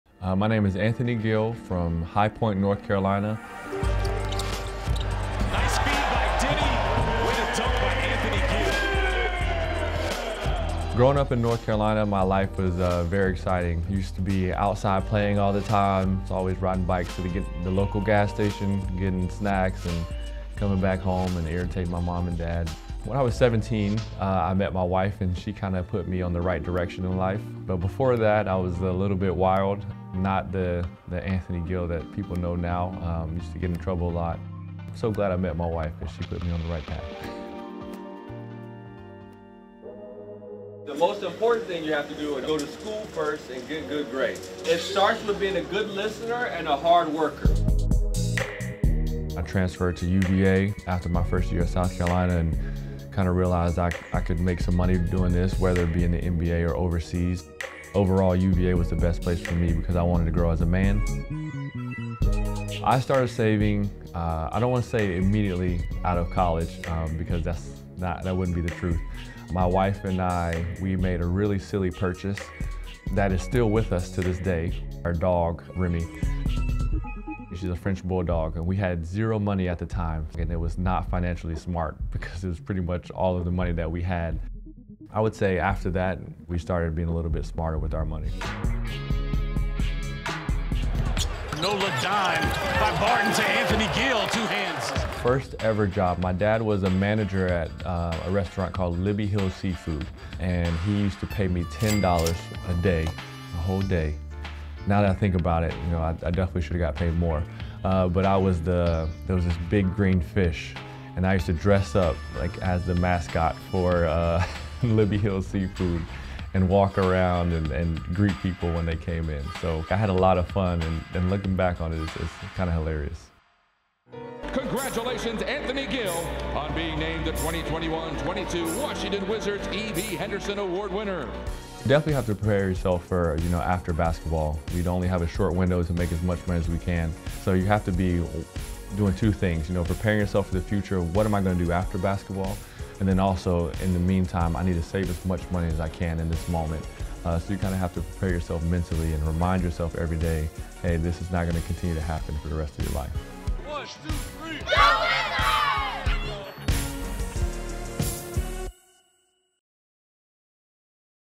Player Profile x Stackwell | Anthony Gill
Anthony Gill talks about the importance of not only preparing for life in the NBA, but preparing for life after basketball.